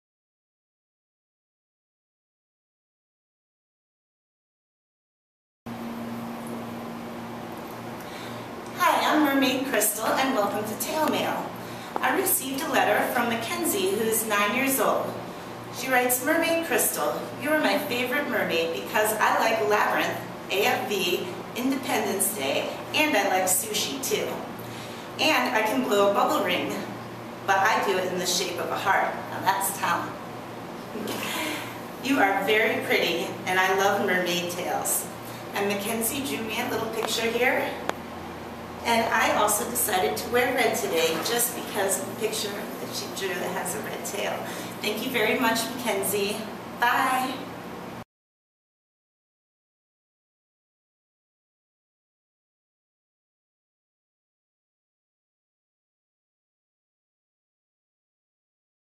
"Tail Mail" for Weeki Wachee Mermaid Crystal from McKenzie in Lexington, SC
Thanks for the letter McKenzie! Tail Mail is a literacy program designed for kids, encouraging them to use their handwriting skills by writing a letter to their favorite mermaid. For more information about sending Tail Mail, visit our web